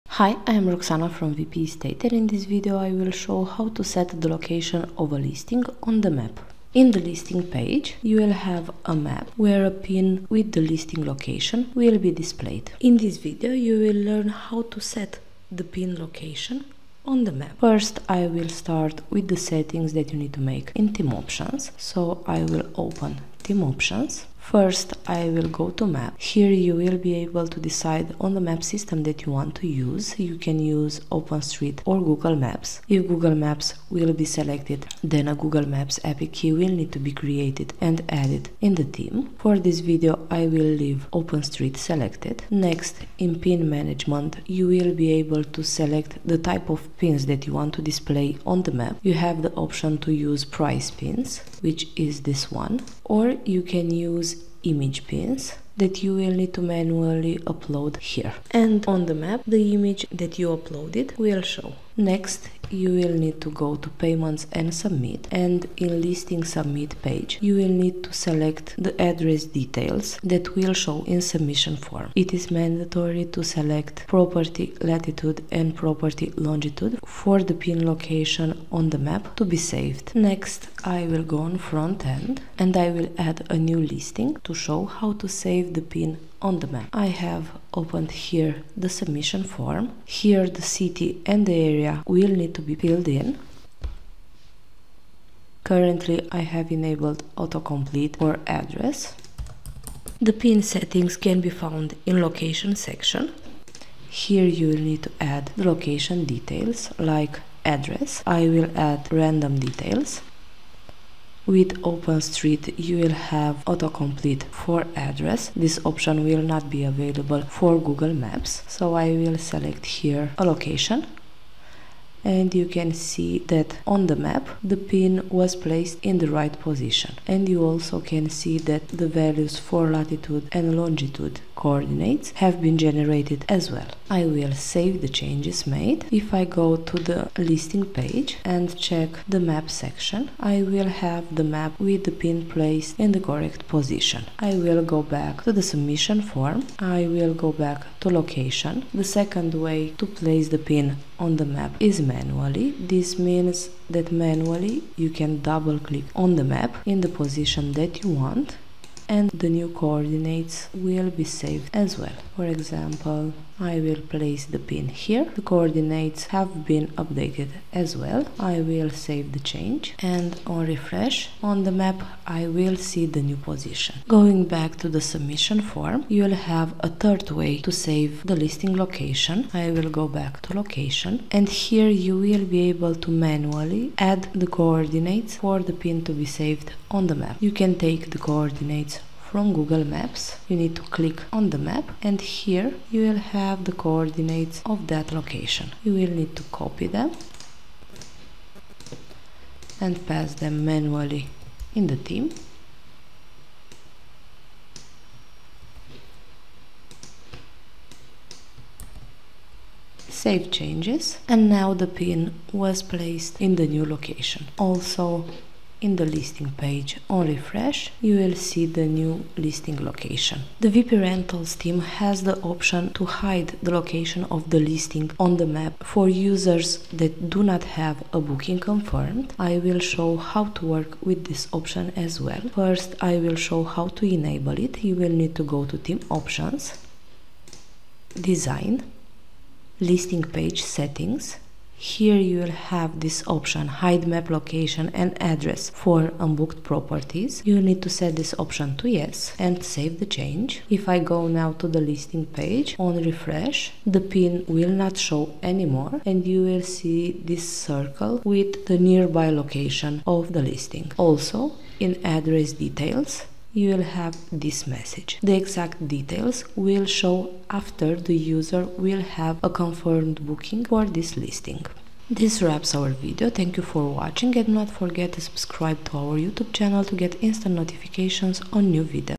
How to set the location of a property on the map in WP Rentals
In this video, you will learn how to set the location of a property on the map in the Wp Rentals theme.